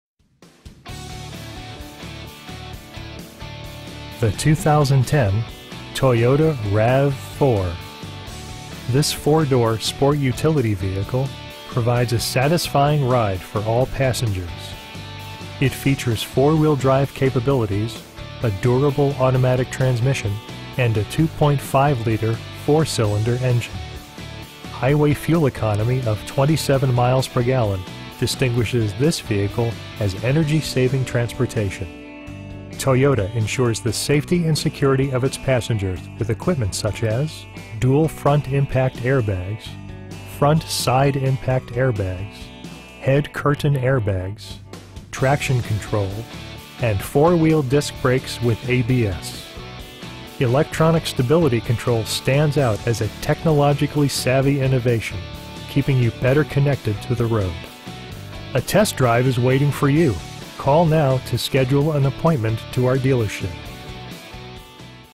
2010 Toyota RAV4 4WD 4-cyl 4-Spd AT in St. Albans, VT 05478
Handy Toyota
39 South Main Street

The 2010 Toyota RAV4. This 4 door sport utility vehicle provides a satisfying ride for all passengers. It features four-wheel drive capabilities, a durable automatic transmission, and a 2.5 liter 4 cylinder engine. Highway fuel economy of 27 miles per gallon distinguishes this vehicle as energy saving transportation. Toyota ensures the safety and security of its passengers with equipment such as: Dual front impact airbags, Front SIDE impact airbags, HEAD curtain airbags, Traction control, and 4 wheel disc brakes with ABS. Electronic stability control stands out as a technologically savvy innovation, keeping you better connected to the road. A test drive is waiting for you.